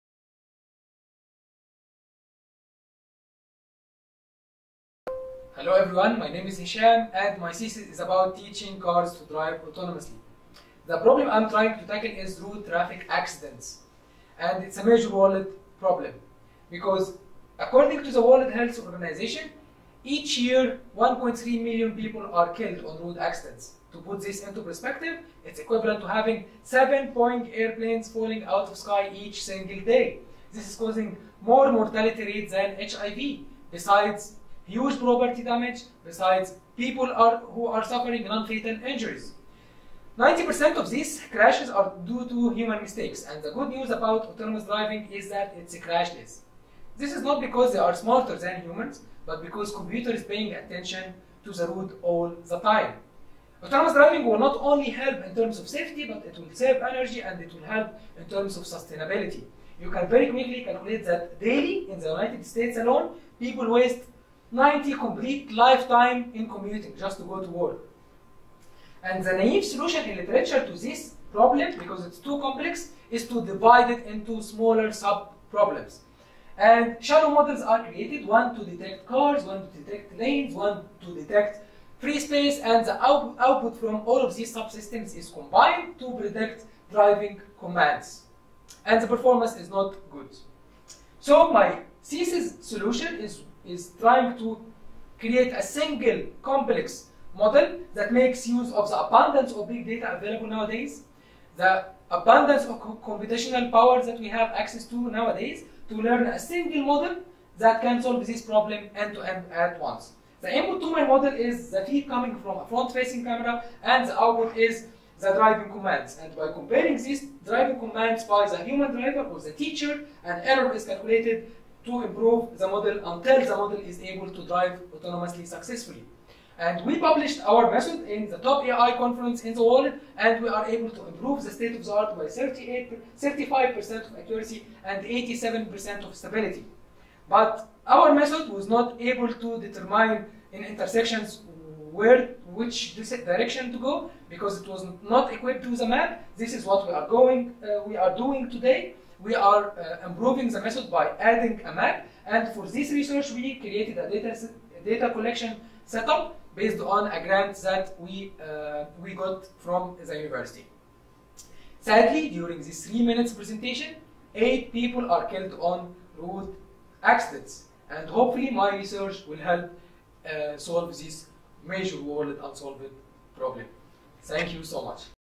Three Minute Thesis (3MT) 2018 Winner - Hesham Eraqi
The Three Minute Thesis (3MT) is an annual research communication competition which challenges research higher degree students to present a compelling oration on their thesis and its significance in just three minutes in language appropriate to a non-specialist audience.
The competition is founded by by University of Queensland.

Hesham Eraqi, Computer Science & Engineering Department was the winner of the American University in Cairo's 2018 competition. He spoke about his research on teaching cars to drive autonomously.